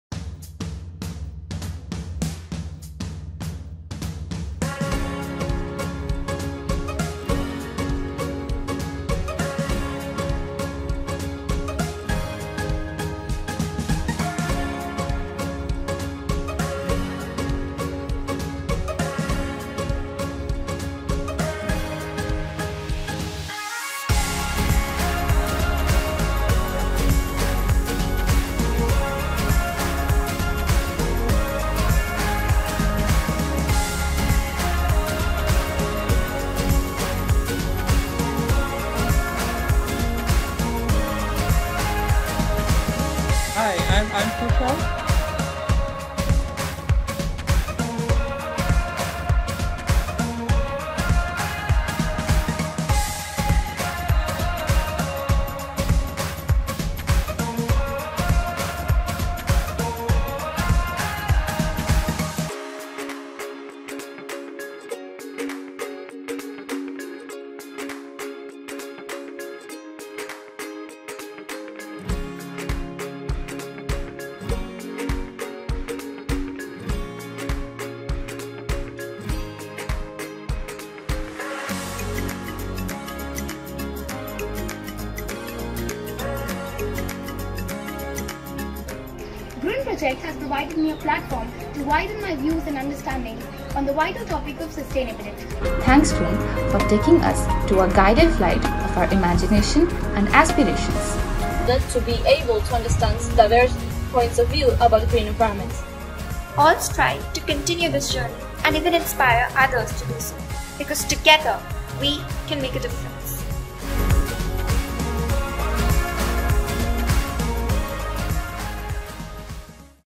GRUEN - Green Urban Environments
GRUEN: Green Urban Environments is a project for secondary school students in Hong Kong, Southeast Asia and Europe that has internationalisation at its heart. GRUEN is co-funded by the Erasmus+ Jean Monnet Programme of the European Union and organized by the Hong Kong Baptist University in Hong Kong SAR.

Under GRUEN, students use a specially-designed online Moodle workshop to participate in asynchronous online discussions and share their perspectives, exchange ideas, and develop their concepts for a green urban environment while also familiarizing themselves with key sustainability principles and best practices of the EU under the 7th EAP.

Working in small cross-cultural groups with like-minded students from schools in Hong Kong, Southeast Asia and Europe, group members collaborate on building their vision for a more sustainable and a healthier living environment which addresses climate change issues, transportation, housing and work conditions as part of the urgent need to combat our negative impact on the environment, among others. Each group is mentored and coordinated by local teachers and the GRUEN team.

At the EU-Asia Schools Conference on Sustainable Cities & Urban Innovation, which is the concluding stage of GRUEN, finalists create group presentations to highlight their innovative visions for a green urban environment. A panel of experts in different areas of sustainability judges all presentations and selects the winners.

The Conference offers a unique opportunity for knowledge exchange between the cross-cultural teams, scholars in the field of sustainability, and the NGO community, while also stimulating further discussion and encouraging the preservation of the world’s natural resources by adopting a greener lifestyle.

The Principal Investigator of GRUEN is Dr. Tushar Chaudhuri, External Relations Coordinator for the Faculty of Social Sciences and Senior Lecturer at the Department of Government & International Studies at Hong Kong Baptist University.

The workshop moderator and technical consultant is Dr. Brant Knutzen, EdD.  The project administrators are Ivelina Ivanova and Jasie TK Lam.